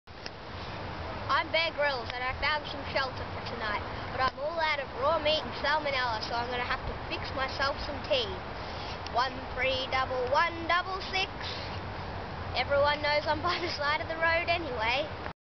Caleb plays Man vs Wild / Bear Grylls, Part 6 of 6
For Calebs 10th birthday he impersonated Bear Grylls, in this video he finds shelter but hasn't a meal yet